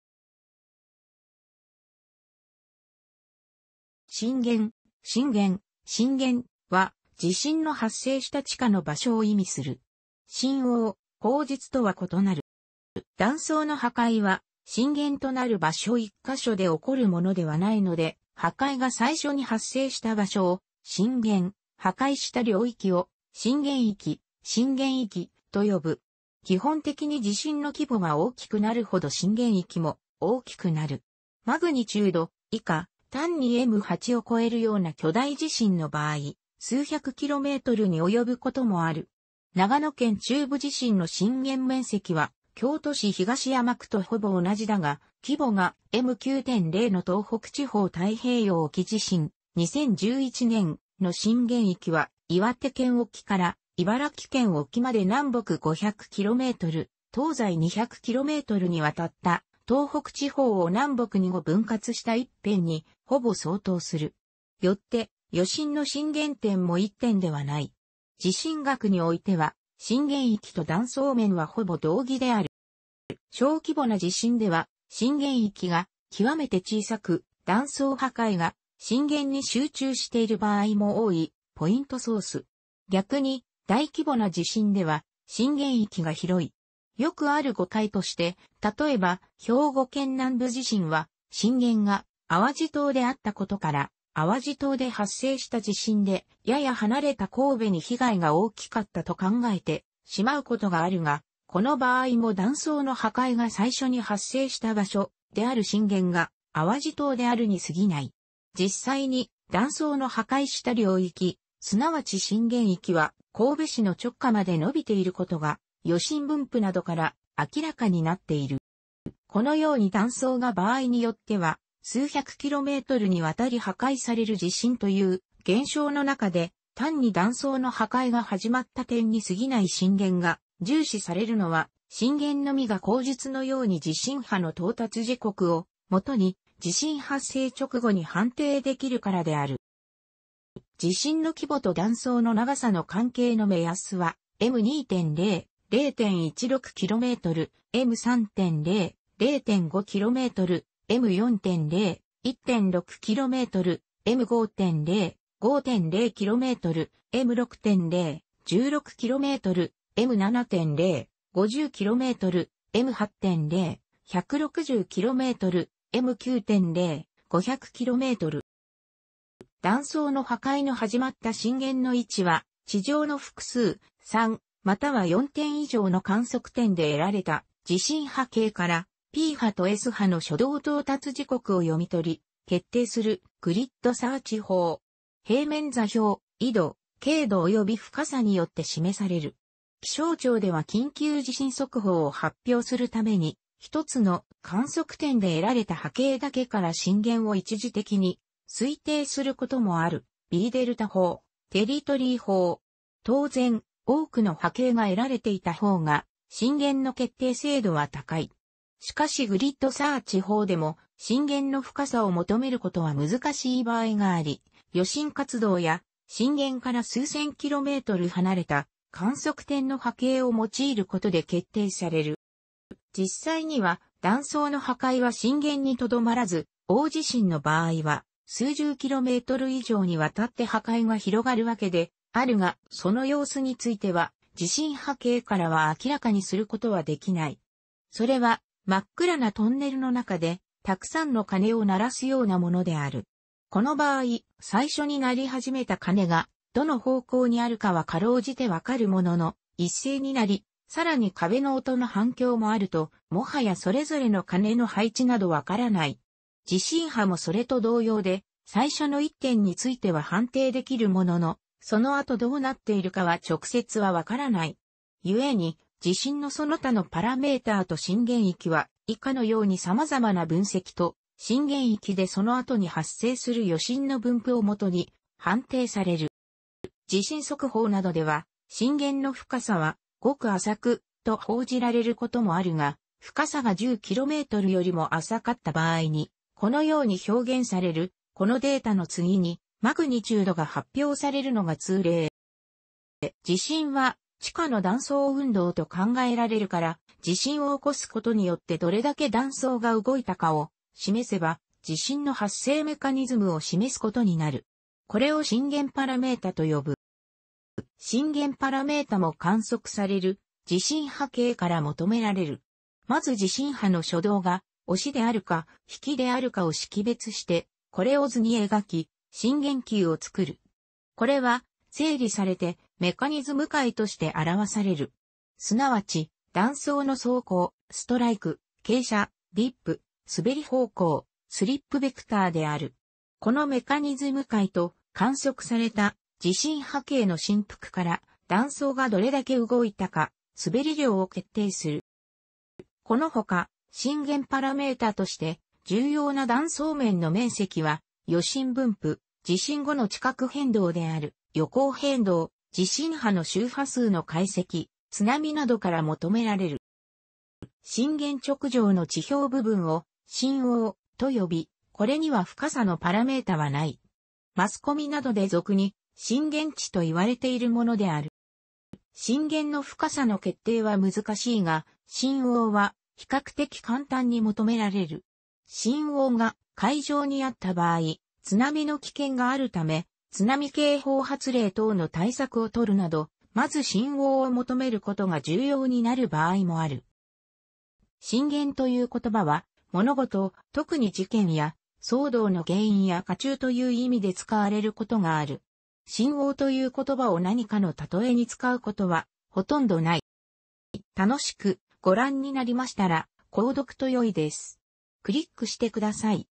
震源
震源（しんげん、）は、地震の発生した地下の場所を意味する。震央（後述）とは異なる。

断層の破壊は震源となる場所1か所で起こるものではないので、破壊が最初に発生した場所を震源、破壊した領域を震源域（しんげんいき、）とよぶ。基本的に地震の規模が大きくなるほど震源域も大きくなる。マグニチュード（以下単にM）8を超えるような巨大地震の場合、数百kmにおよぶこともある。長野県中部地震の震源面積は京都市東山区とほぼ同じだが、規模がM9.0の東北地方太平洋沖地震（2011年）の震源域は岩手県沖から茨城県沖まで南北500 km、東西200 kmにわたった（東北地方を南北に5分割した一片にほぼ相当する）。よって余震の震源点も一点ではない。地震学においては、震源域と断層面はほぼ同義である。

小規模な地震では震源域が極めて小さく、断層破壊が震源に集中している場合も多い（ポイントソース）。逆に大規模な地震では震源域が広い。よくある誤解として、例えば兵庫県南部地震は震源が淡路島であったことから、"淡路島で発生した地震で、やや離れた神戸に被害が大きかった"と考えてしまうことがあるが、この場合も"断層の破壊が最初に発生した場所"である震源が淡路島であるにすぎない。実際に断層の破壊した領域、すなわち震源域は神戸市の直下まで伸びていることが、余震分布などから明らかになっている。

このように断層が場合によっては数百 kmにわたり破壊される地震という現象の中で、単に断層の破壊が始まった点にすぎない震源が重視されるのは、震源のみが後述のように地震波の到達時刻をもとに、地震発生直後に判定できるからである。

地震の規模と断層の長さの関係の目安は、M2.0：0.16 km、M3.0：0.5 km、M4.0：1.6 km、M5.0：5.0 km、M6.0：16 km、M7.0：50 km、M8.0：160 km、M9.0：500 km。

断層の破壊の始まった震源の位置は、地上の複数（3または4点以上）の観測点で得られた地震波形から、P波とS波の初動到達時刻を読み取り、決定する（グリッドサーチ法）。平面座標（緯度・経度）及び深さによって示される。気象庁では緊急地震速報を発表するために、1つの観測点で得られた波形だけから震源を一時的に推定することもある（BΔ法、テリトリー法）。当然、多くの波形が得られていたほうが震源の決定精度は高い。しかしグリッドサーチ法でも震源の深さを求めることは難しい場合があり、余震活動や、震源から数千 km離れた観測点の波形を用いることで決定される。

実際には、断層の破壊は震源にとどまらず、大地震の場合は数十 km以上にわたって破壊が広がるわけであるが、その様子については地震波形からは明らかにすることはできない。それは真っ暗なトンネルの中でたくさんの鐘を鳴らすようなものである。この場合、最初に鳴りはじめた鐘がどの方向にあるかは辛うじて分かるものの、一斉に鳴り、さらに壁の音の反響もあると、もはやそれぞれの鐘の配置など分からない。地震波もそれと同様で、最初の1点については判定できるものの、その後どうなっているかは直接は分からない。故に地震のその他のパラメーターと震源域は、以下のように様々な分析と、震源域でその後に発生する余震の分布をもとに判定される。

地震速報などでは「震源の深さはごく浅く……」と報じられる事もあるが、深さが10 kmよりも浅かった場合にこのように表現される（このデータの次にマグニチュードが発表されるのが通例）。

地震は地下の断層運動と考えられるから、地震を起こすことによってどれだけ断層が動いたかを示せば、地震の発生メカニズムを示すことになる。これを震源パラメータと呼ぶ。

震源パラメータも観測される地震波形から求められる。まず地震波の初動が「押し」であるか「引き」であるかを識別してこれを図に描き、震源球を作る。これは整理されてメカニズム解として表される。すなわち、断層の走向（strike）、傾斜（dip）、すべり方向（slip vector）である。このメカニズム解と、観測された地震波形の振幅から、断層がどれだけ動いたか（すべり量）を決定する。

このほか、震源パラメータとして重要な断層面の面積は、余震分布、地震後の地殻変動である余効変動、地震波の周波数の解析、津波などから求められる。

震源直上の地表部分を震央（）と呼び、これには深さのパラメータはない。マスコミなどで俗に震源地と言われているものである。

震源の深さの決定は難しいが、震央は比較的簡単に求めら...